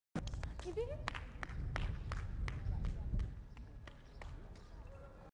International Film & Video
Tourists leave a building in Bukhara, Uzbekistan.